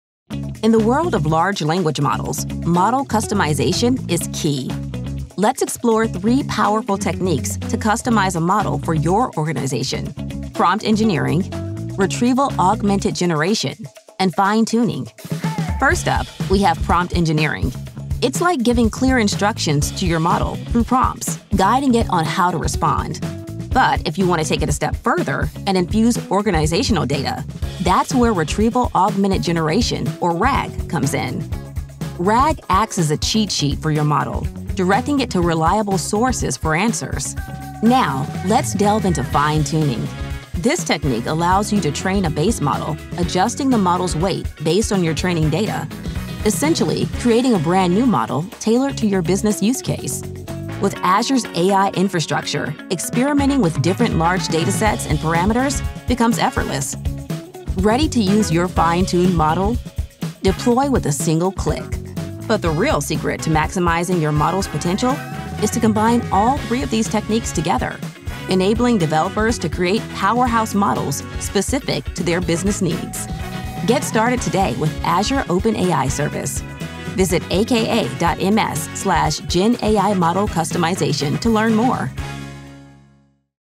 How to customize GenAI models with Azure OpenAI Service
Explore the power of model customization with Azure's OpenAI Service as we uncover three techniques: Prompt Engineering, Retrieval Augmented Generation (RAG), and Fine-tuning. Learn how to tailor AI models to your business needs effortlessly and revolutionize your approach to AI.